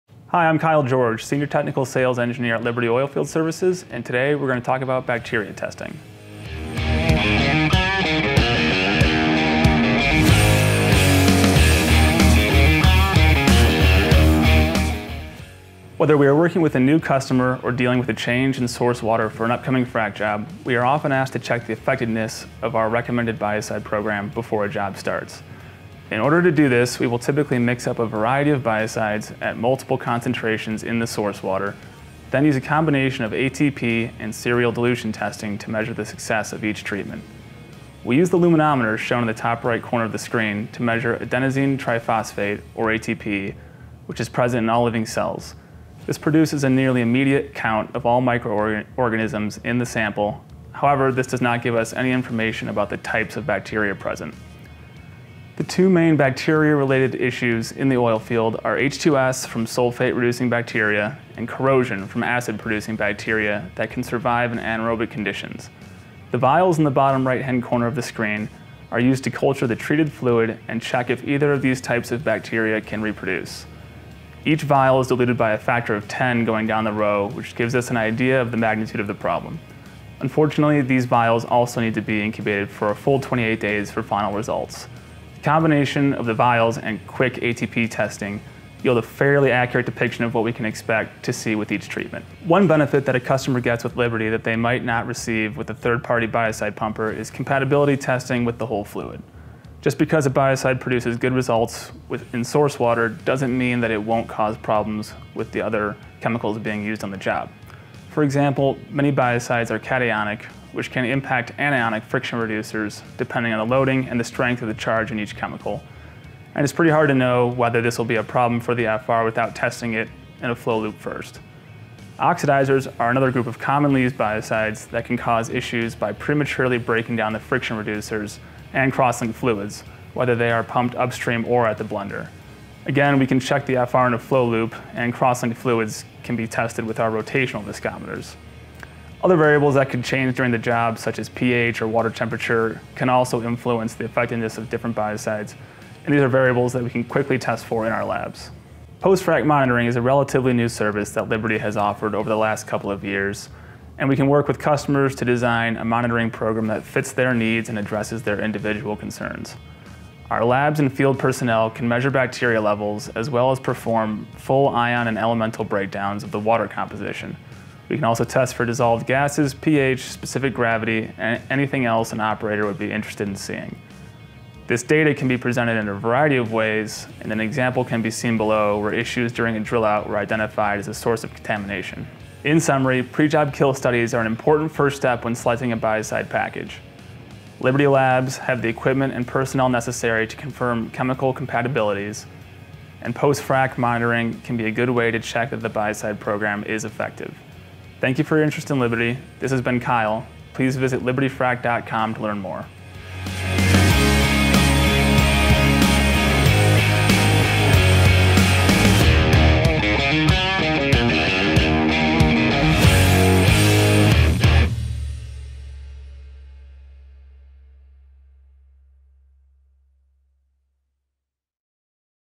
Bacteria Testing & Monitoring
Presence of bacteria can have a long-term impact on reservoir performance.  Liberty Sr. Staff Engineer Kyle George explains that Liberty can monitor for bacteria during and after the job to minimize their impact and help our customers make the right choices for biocides.